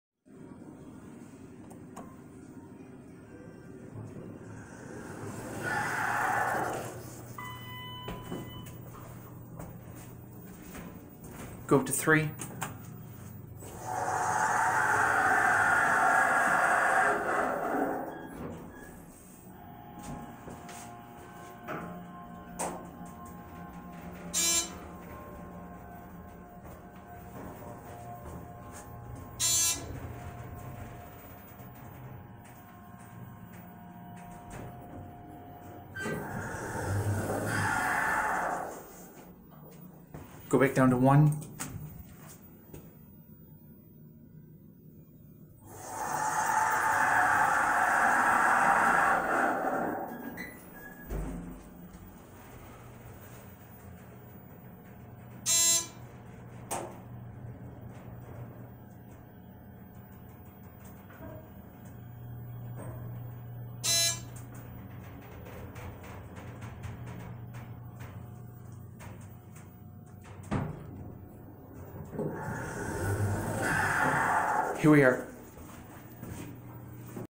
ThyssenKrupp Signa4 Hydraulic Elevator at Holiday Inn Express & Suites, Bay City, MI
Riding the basic elevator at Holiday Inn Express & Suites, in Bay City, MI.

TECHNICAL SPECS:
Make: ThyssenKrupp
Type: Hydraulic
Capacity: 2500 Lbs.
Number of Floors: 3
Year Installed: 2009